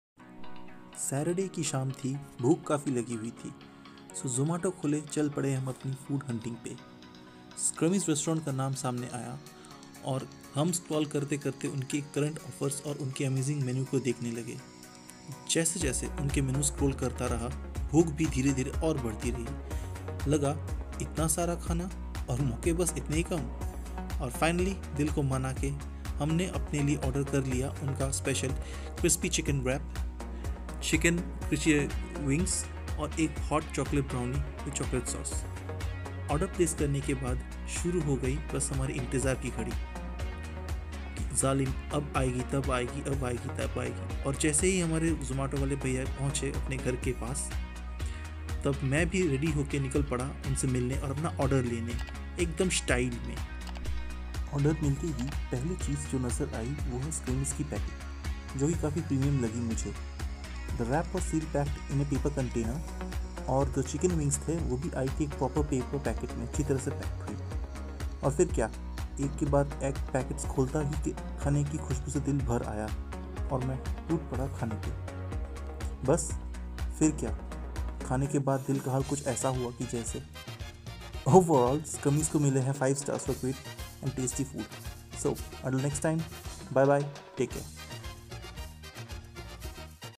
SCRUMMYS Kharagpur🍔|| Best quality crispy chicken wraps, Crispy Chicken Wings for home delivery???
A lazy saturday afternoon post of a hungry enginner looking for a quick bite but not willing to go out

 • Upbeat Reel Backg...